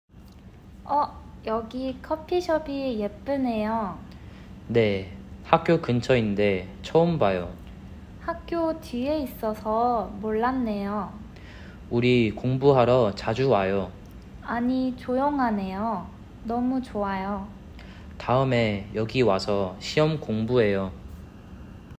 (K102) L12-3 -네요 (spontaneous reactional suffix) (1)
Example 1:

수민: 어, 여기 커피숍이 예쁘네요*. *(예쁘다: pretty)
다니엘: 네, 학교 근처인데 처음 봐요.
수민: 학교 뒤에 있어서 몰랐네요.
다니엘: 우리 공부하러 자주 와요.
수민: 안이 조용하네요. 너무 좋아요.
다니엘: 다음에 여기 와서 시험 공부해요.

Example 1 (with English):

 Oh, this coffee shop is pretty.
 Yes, it's near the school, but it's my first time seeing it.
 I didn't know about it because it’s behind the school.
 We should come here often to study.
 The interior is quiet. It's so nice.
 Let's come here next time to study for exams.